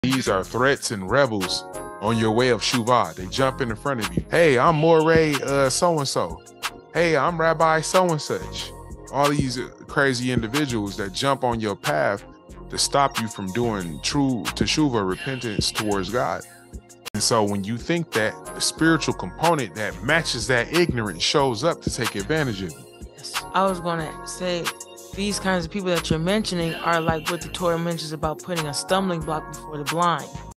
Defeat the Rebels on Your Path to Shuvah✊
Explore the obstacles that hinder true repentance and learn how to overcome them with spiritual strength. Join Moray and Rabbi as they guide you towards a fulfilling and authentic teshuvah journey.